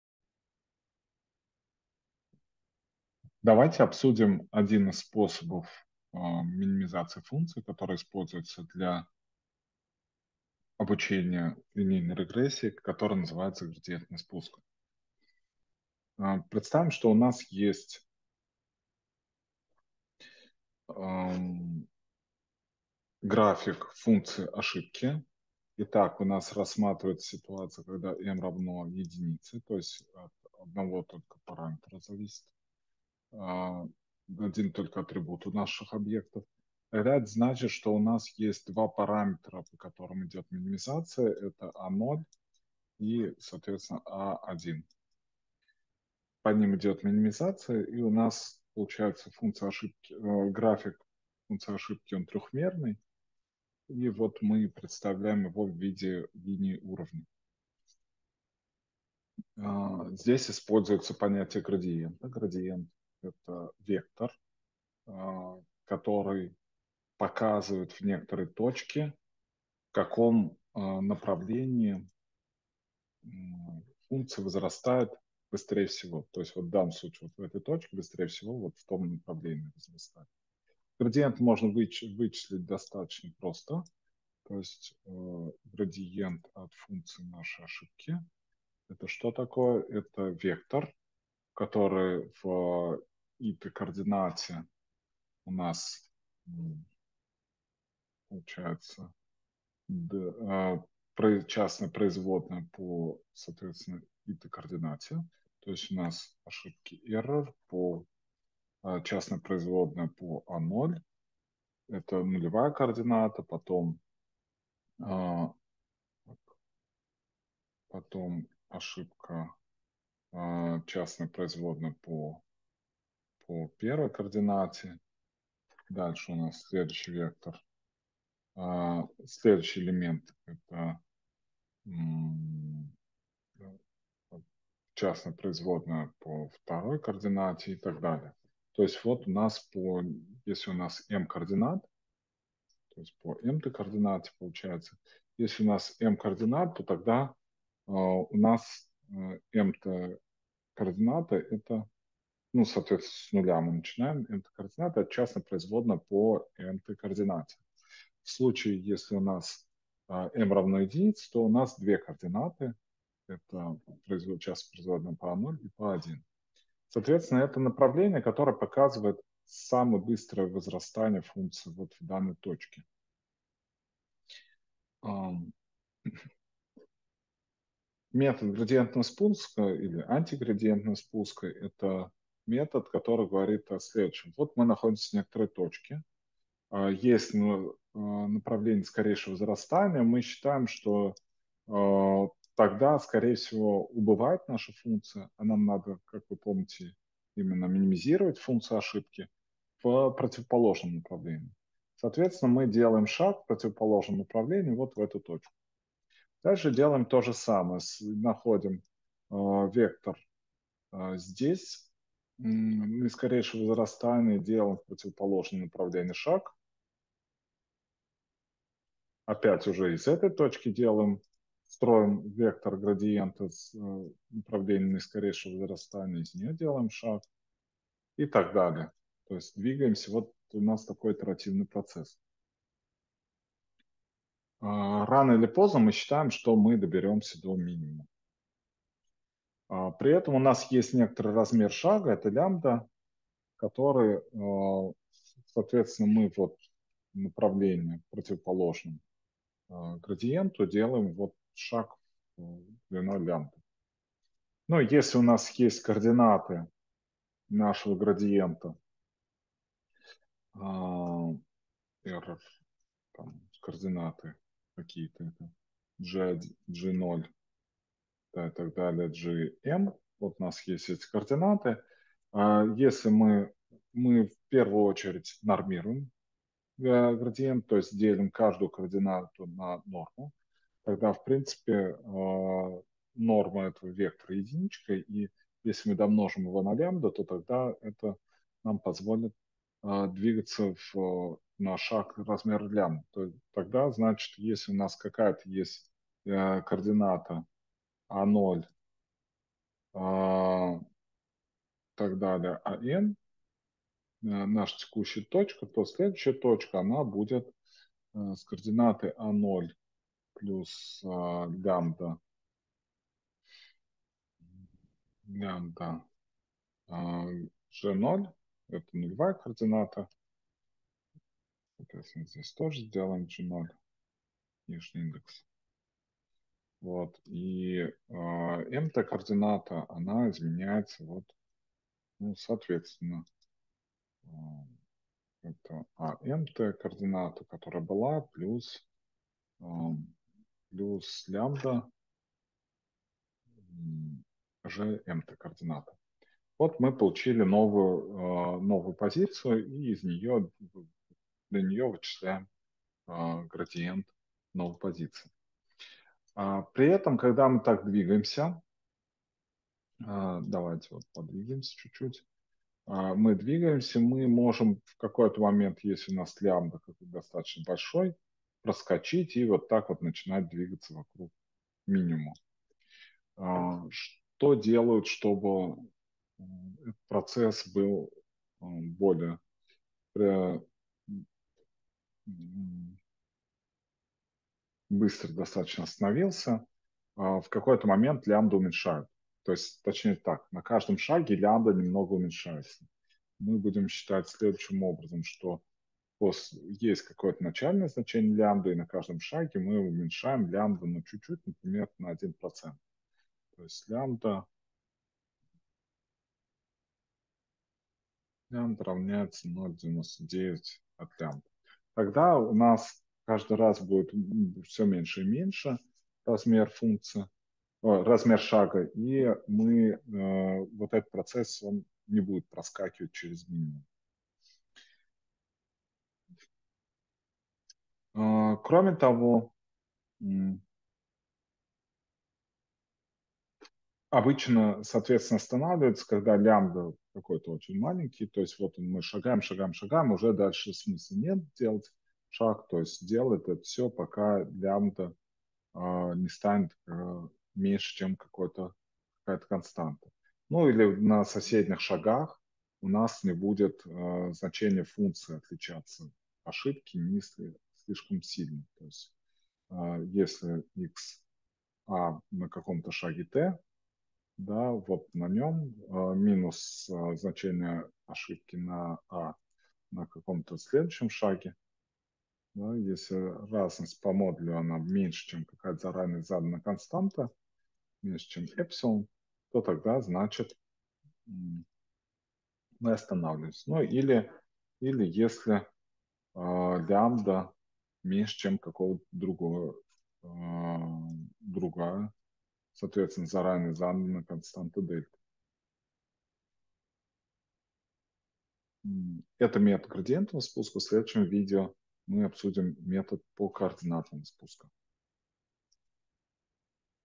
МО. Лекция 1.4 Обучение линейной регрессии. Градиентный спуск
МО. Лекция 1.4 Обучение линейной регрессии. Градиентный спуск или антиградиентный спуск.

Курс Машинное обучение.

Лектор: Хадиев Камиль, Казанский федеральный университет.